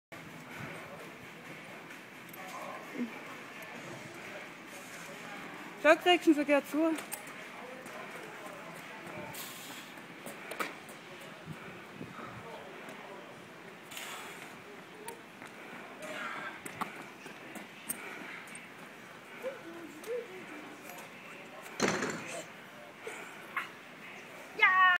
Koffer tragen - Farmers Walk 85kg - Strongman Training / Workout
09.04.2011
Koffer tragen 85kg
Farmers Walk 85kg
Mathias Nesensohn